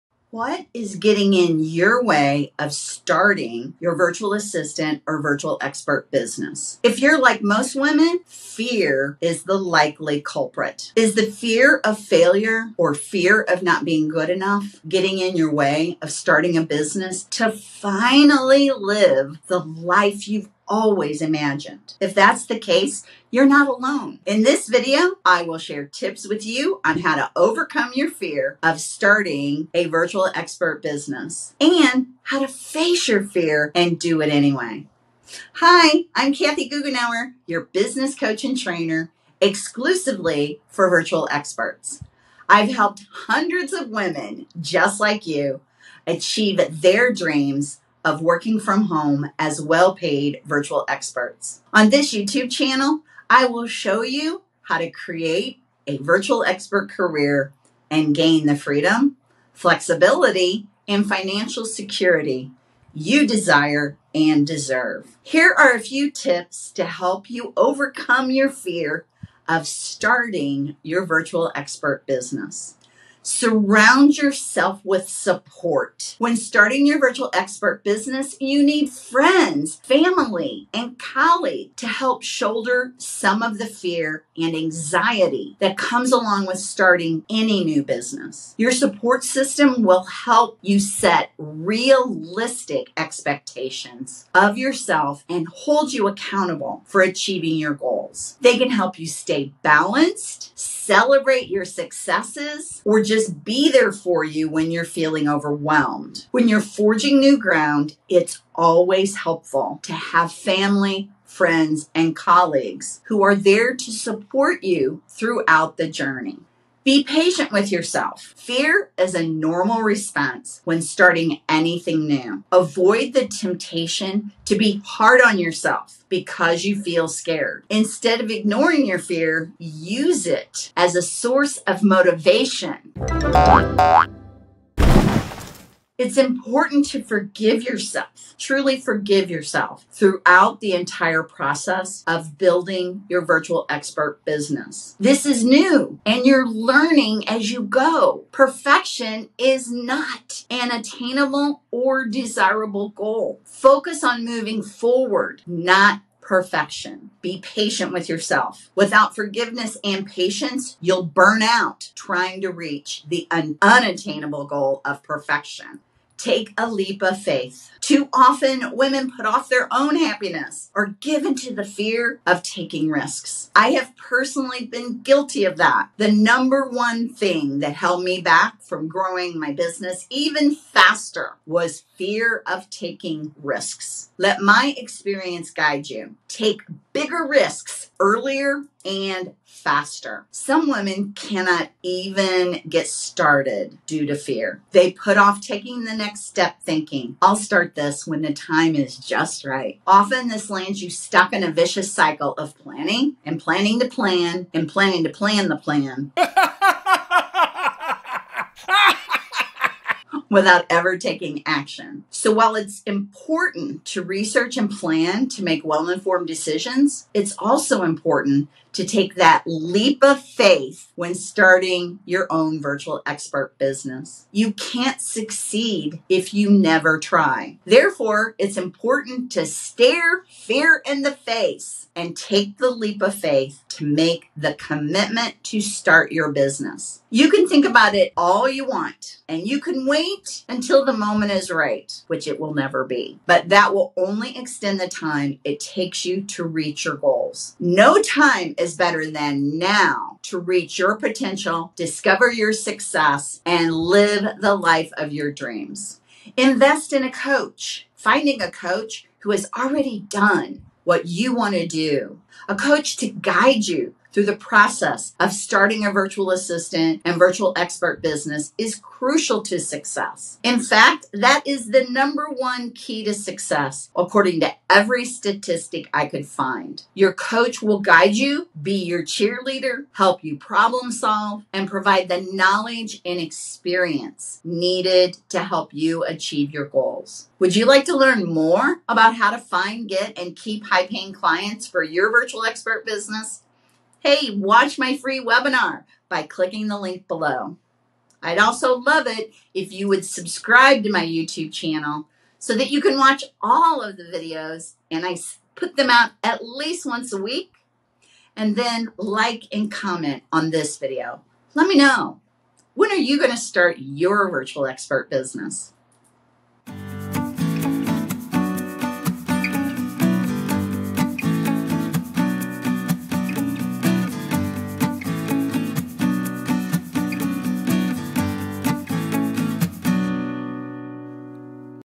How to Get Over Your Fear of Starting a Virtual Assistant Business|Kathy Goughenour
In this video, I will share with you tips on how to overcome your fear of starting a VE business. And how to face your fear and do it anyway.

What is getting in your way of starting your Virtual Assistant or Virtual Expert business? If you’re like most women, fear is the likely culprit.

Is the fear of failure or fear of not being good enough getting in your way of starting a business to finally live the life you’ve always imagined? You’re not alone.

“The Business of Being in the Virtual Assistant Business: 5 Expert Essentials”

🔗LET’S CONNECT!